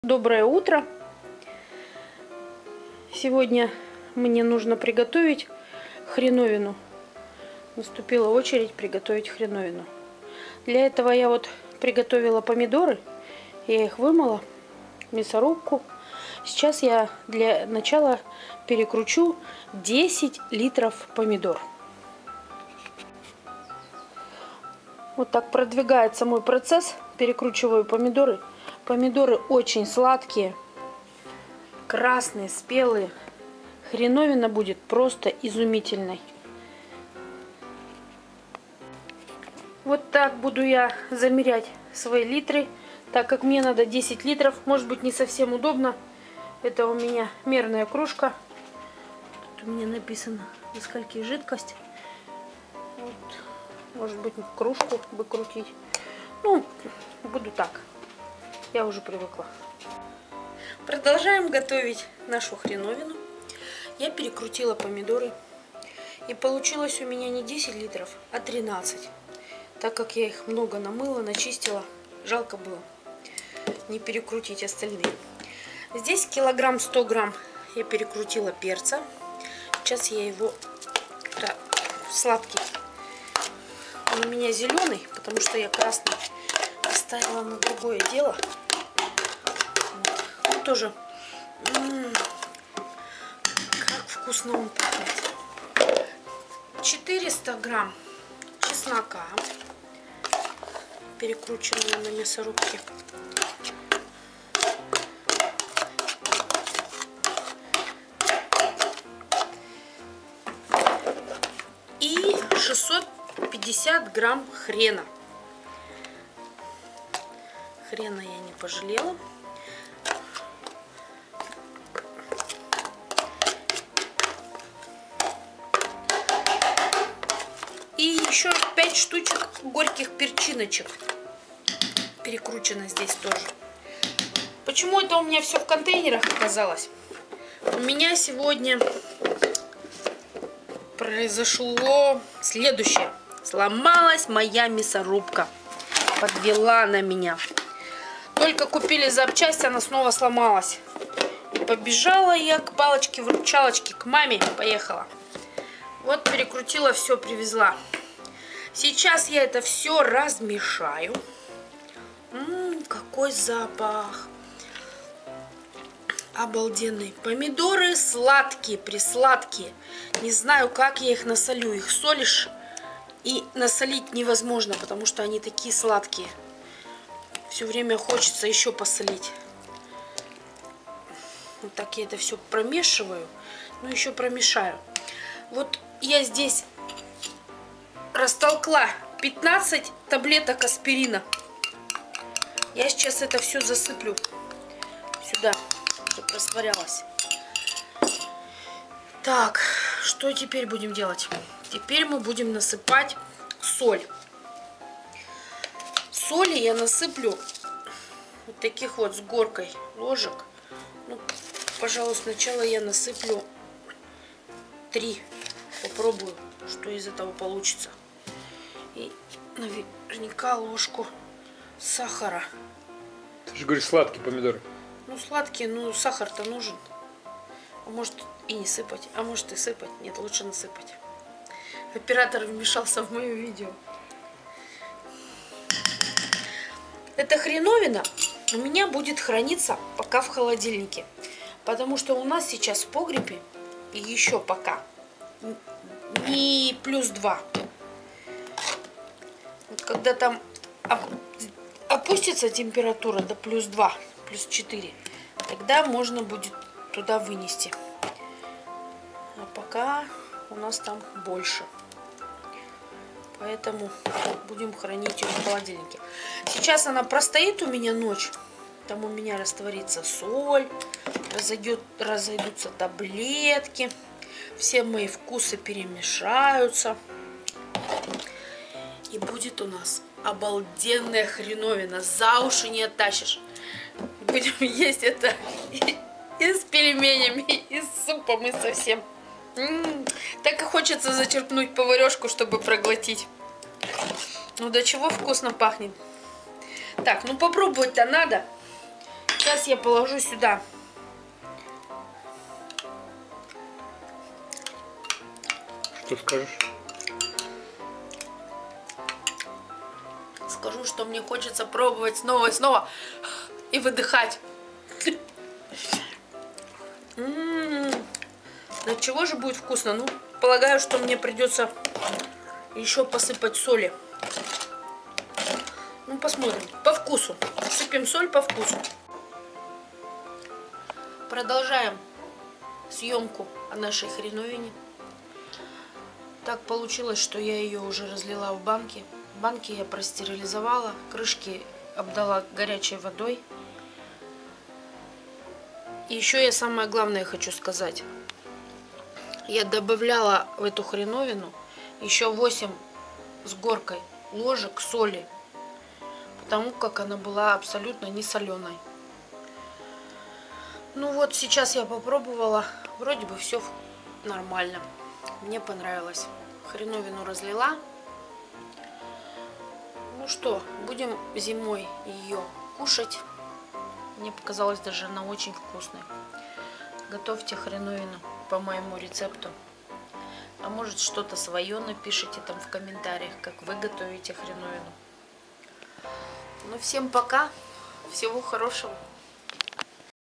Хреновина на зиму! Очень вкусная и полезная! Plant horseradish ! Very tasty!
Хреновину по этому рецепту готовим очень давно! Так готовить хреновину на зиму меня научила моя мама!
  Кладу аспирин, чтобы не бродила , так как эта хреновина находится всю зиму под пластиковой крышкой. Она, хреновина ,  не стерилизована, то есть все витамины сохранены по максимуму!
   Хреновину для вкуса и полезности, готовлю обязательно из поспевших на кусту помидоров, томатов!
Удачи вам в приготовлении ХРЕНОВИНЫ!